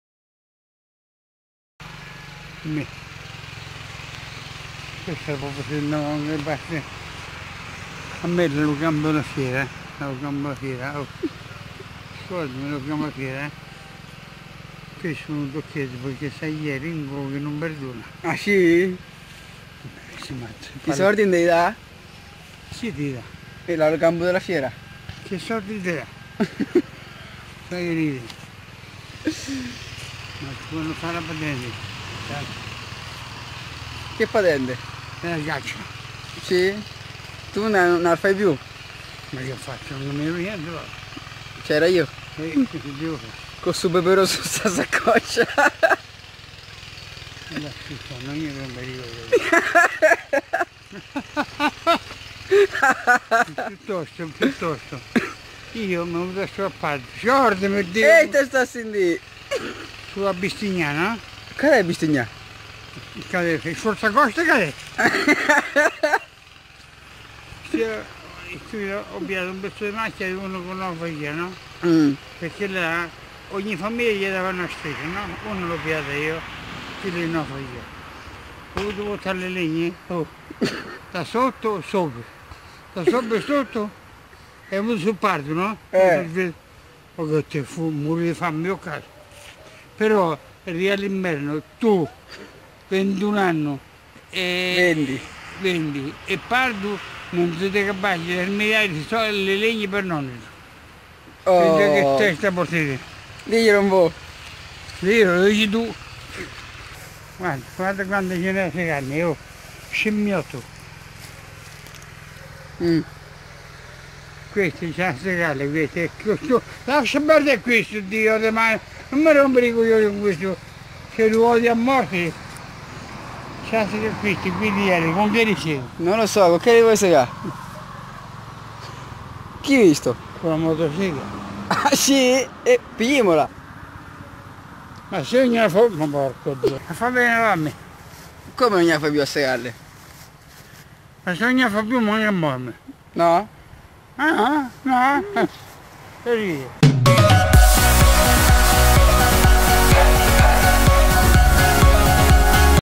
Nonno Renzo,Episodio 2
Episodio 2,nonno renzo storia di un inverno passato che non arriverà mai! in questo video mio nonno esprime i suoi pensieri riguardo all'inverno ed altre cose senza senso con un peperone in tasca che valorizza al massimo la sua figura di agricoltore,se il video vi è piaciuto cliccate mi piace e condividi :D